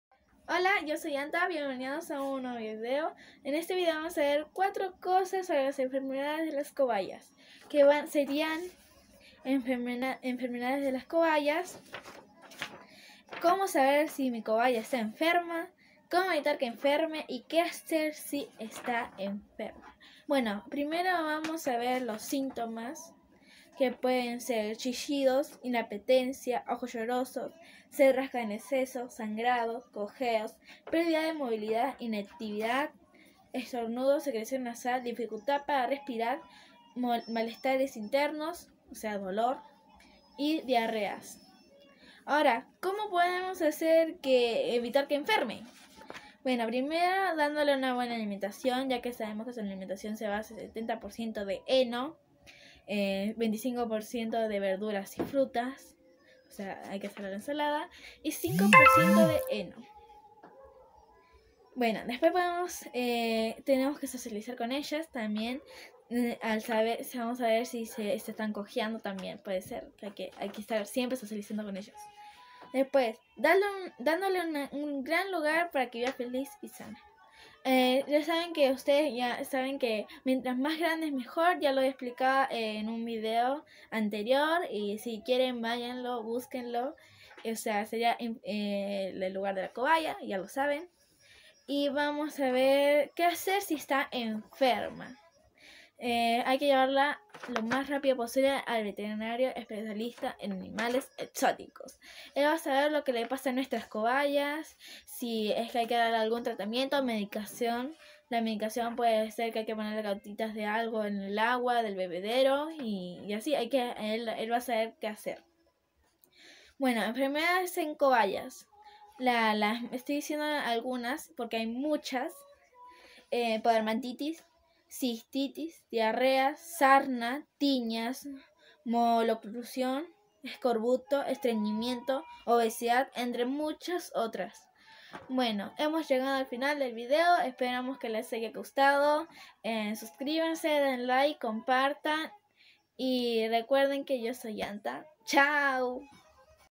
enfermedades de las cobayas , como prevenir,y que hacer
vídeo sobre las enfermedades de las cobayas ,síntomas, como prevenir y que hacer si enferma 🐹❗❗❗